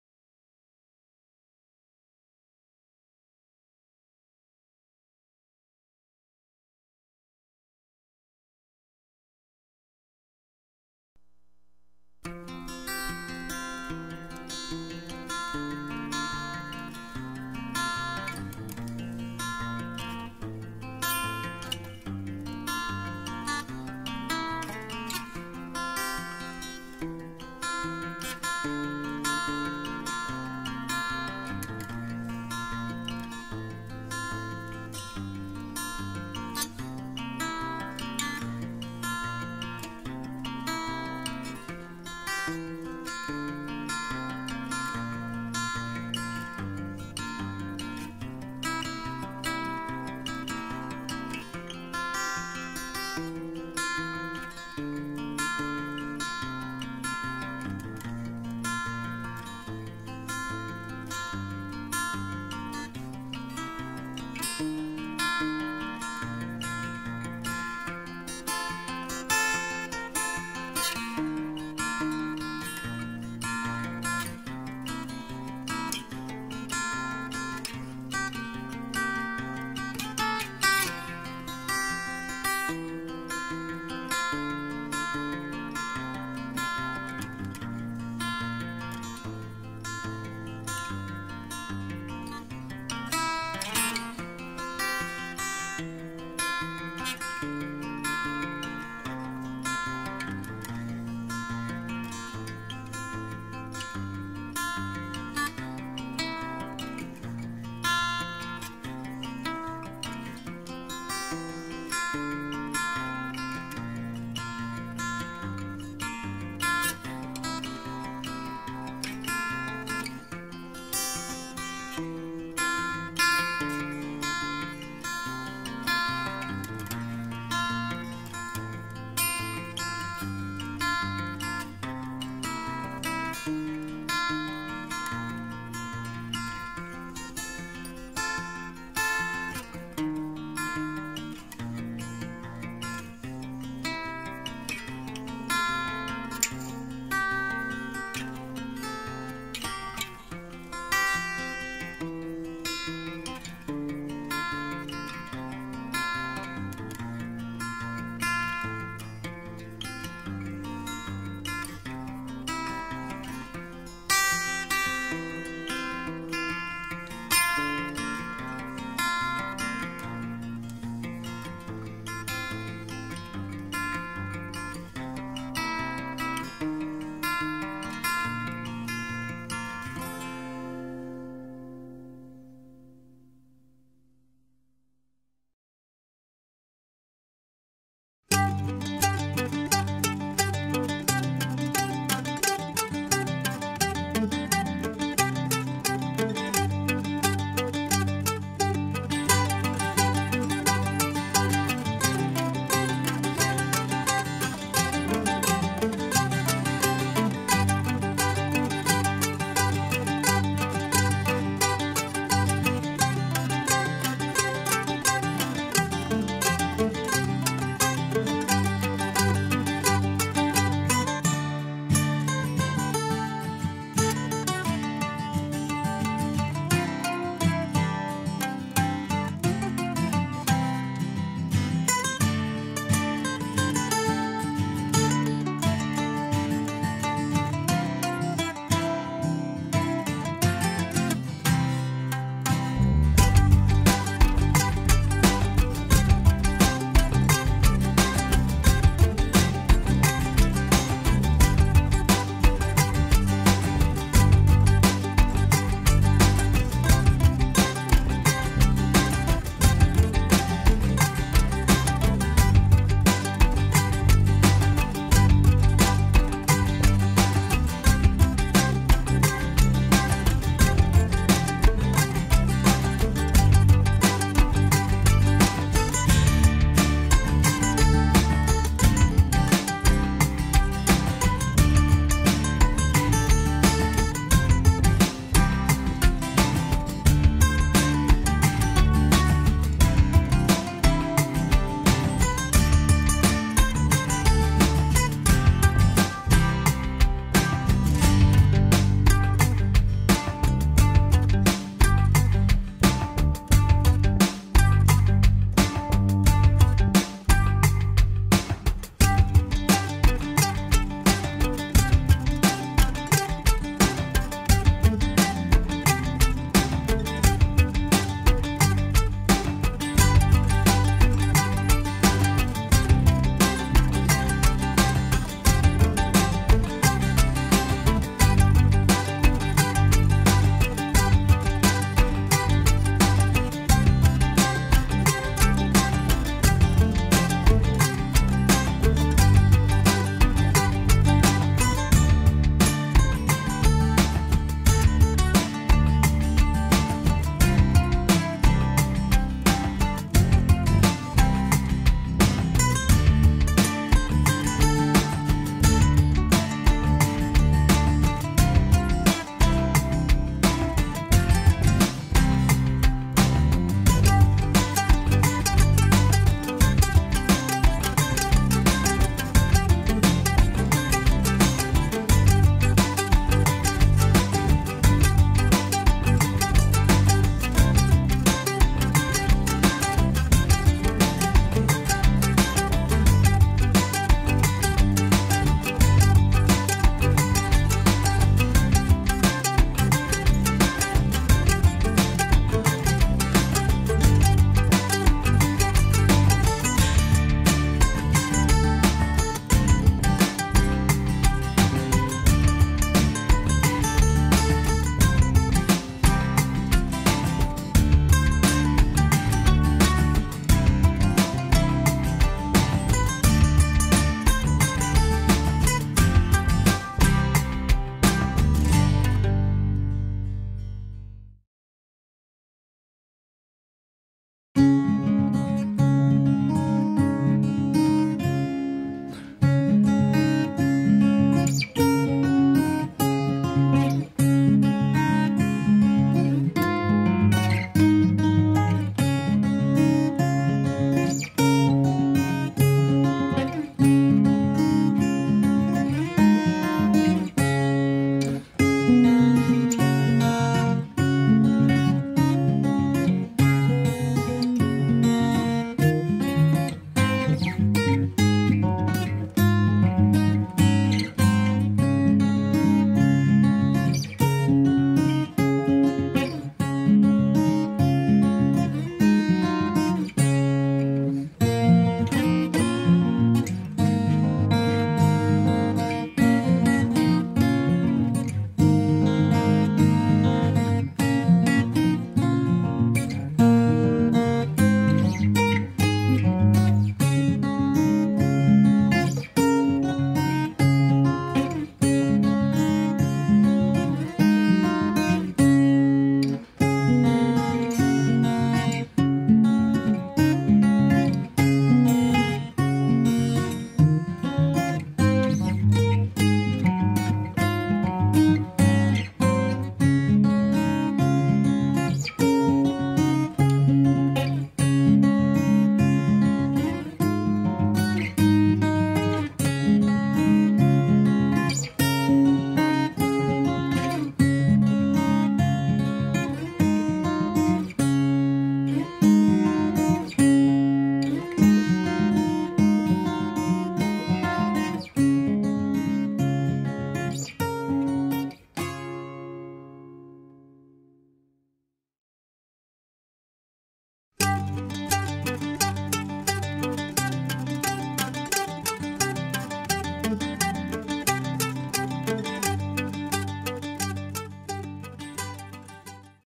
Speedpaint - Raccoon- Watercolor and Gouache
Timelapse video of my latest painting, "Timid".  5 x 5, watercolor and gouache on illustration board.